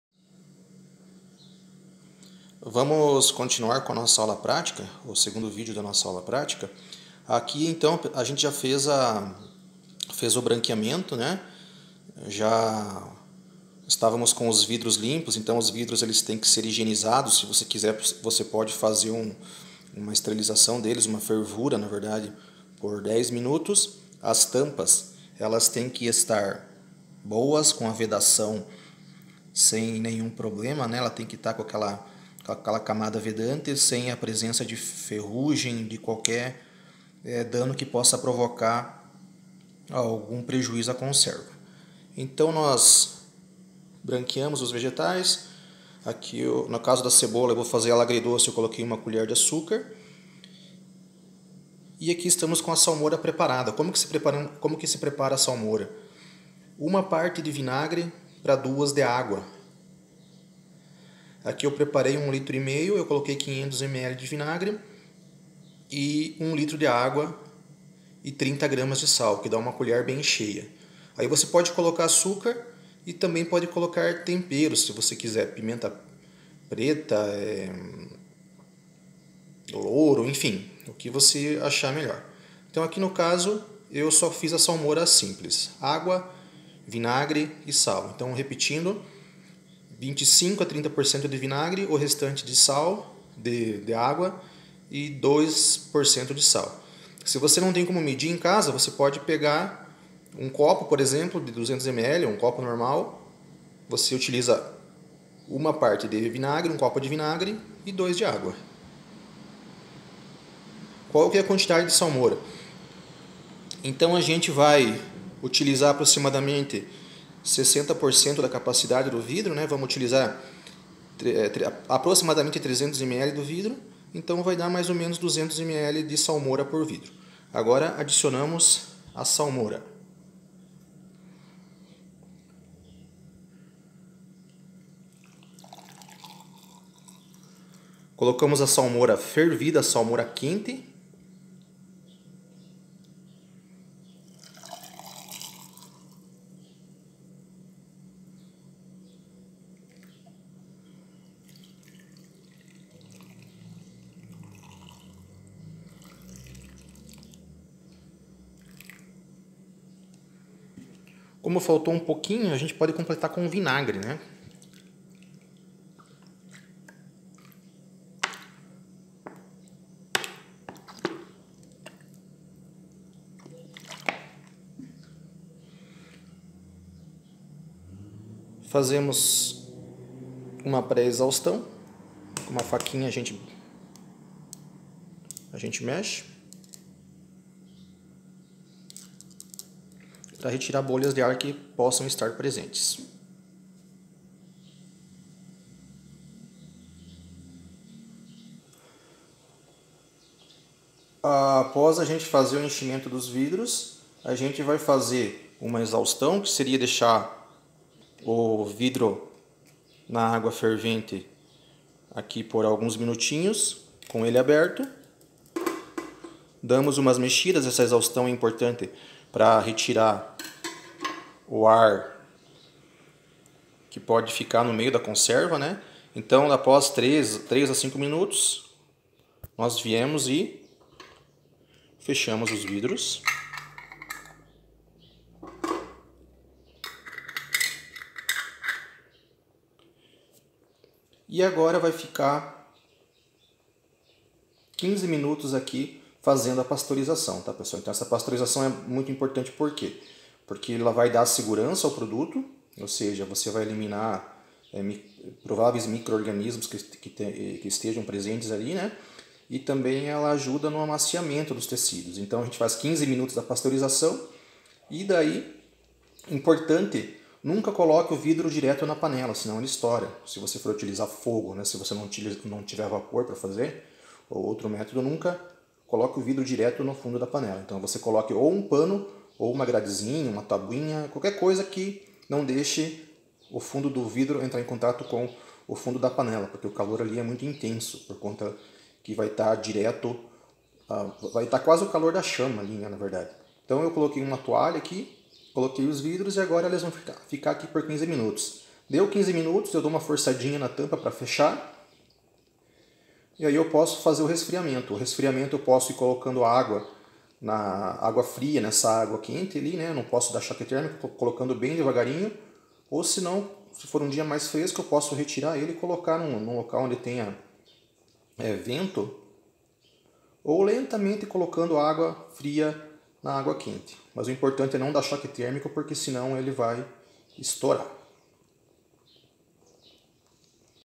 Aula prática Agroindústria CONSERVAS -VIDEO 2
Aula prática fabricação de conservas em salmoura com descrição do processo - video 2 - preparo da salmoura, enchimento dos vidros, exaustão e pasteurização.  Segundos anos Ceep Olegário Macedo - processamento de vegetais